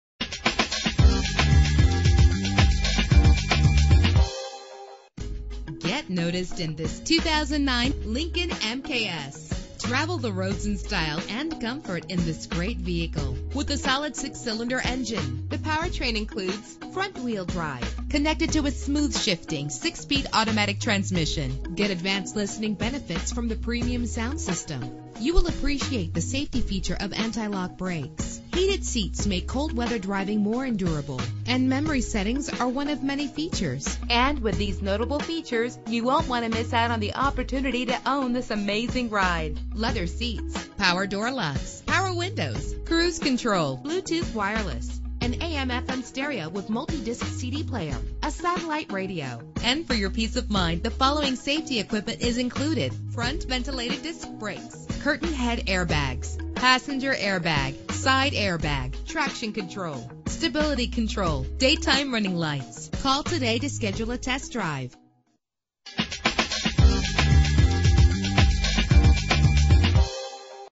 2009 Lincoln MKS - Downersgrove IL
Year: 2009
Make: Lincoln
Model: MKS
Trim: Base
Engine: 3.7 liter 6 cylinder 24 valve
Transmission: 6-Speed Automatic
Color: Dark Blue Ink Clearcoat Metallic
Mileage: 48654
Address:
2311 West Ogden